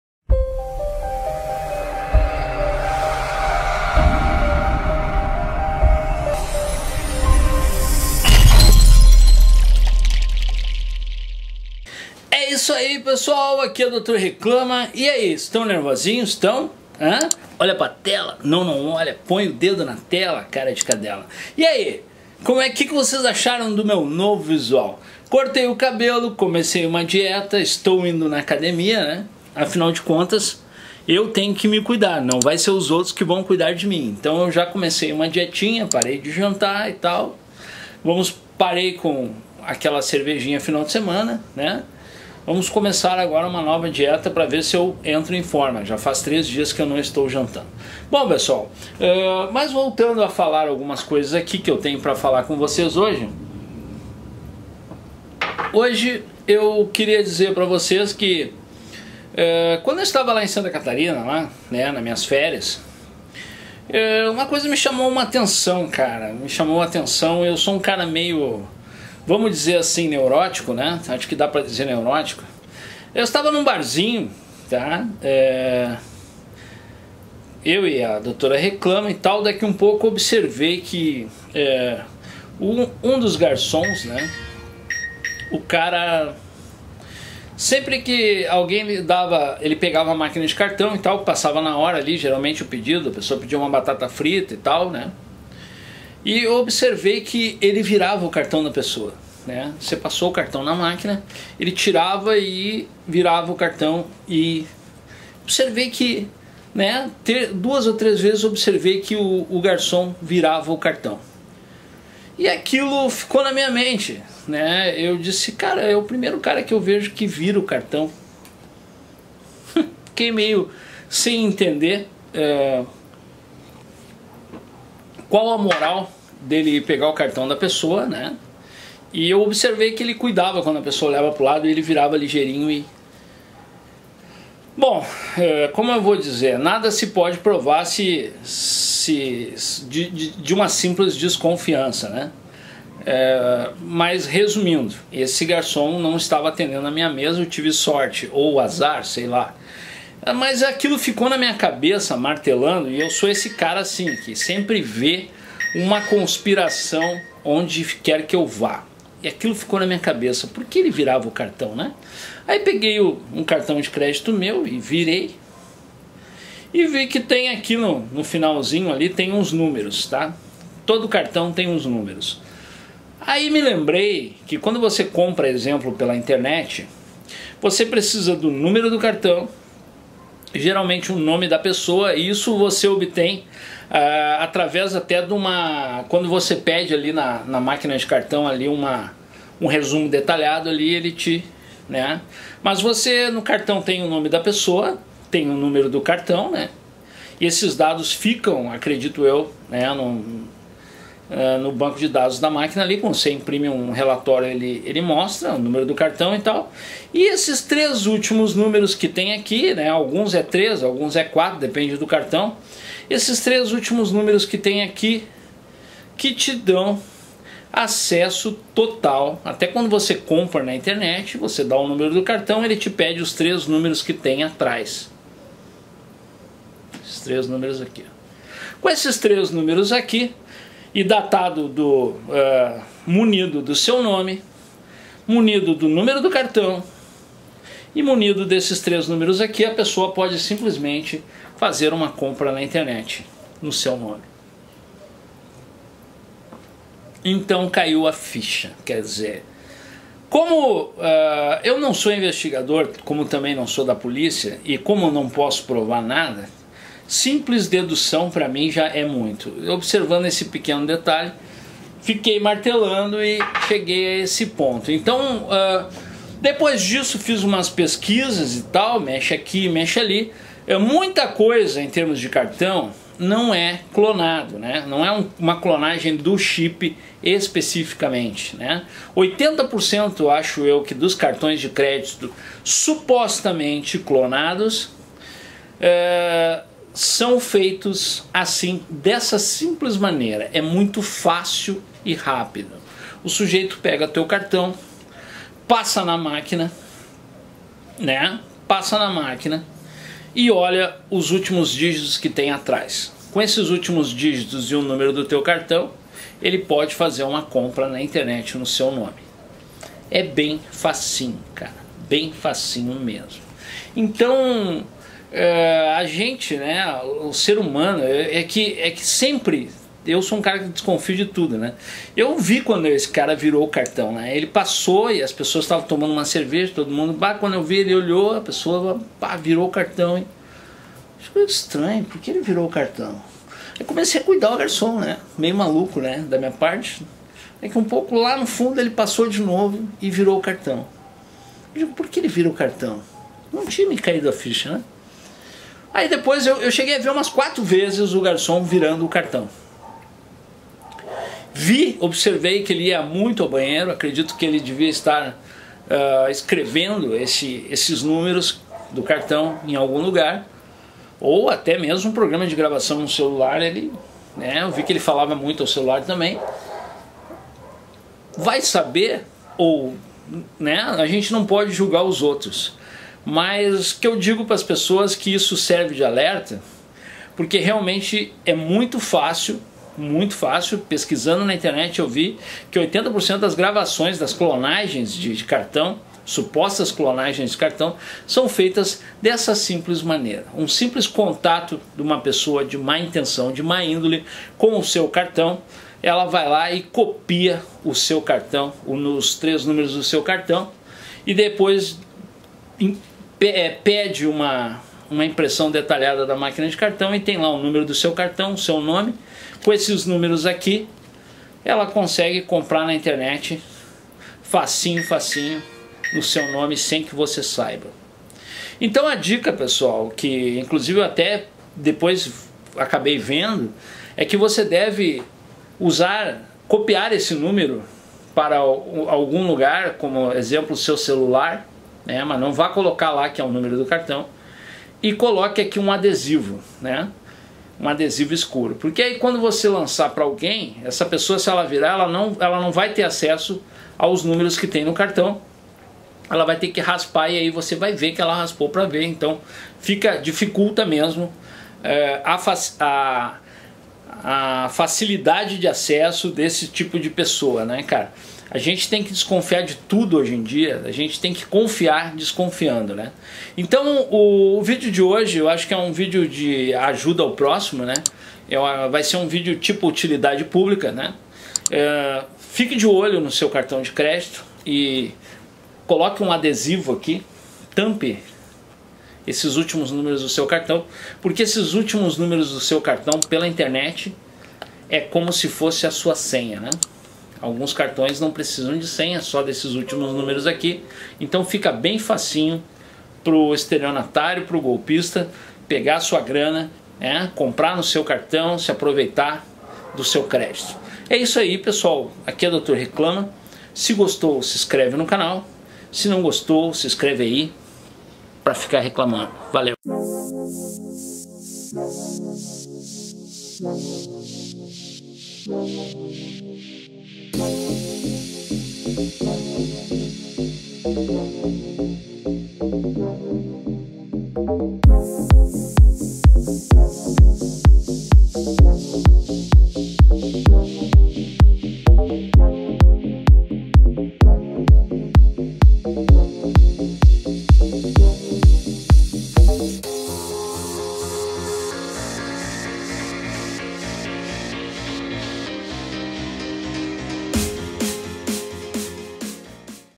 BOMBA SAIBA COMO CLONAM SEU CARTÃO DE CRÉDITOS
saiba como se prevenir um golpe que e mais praticado no Brasil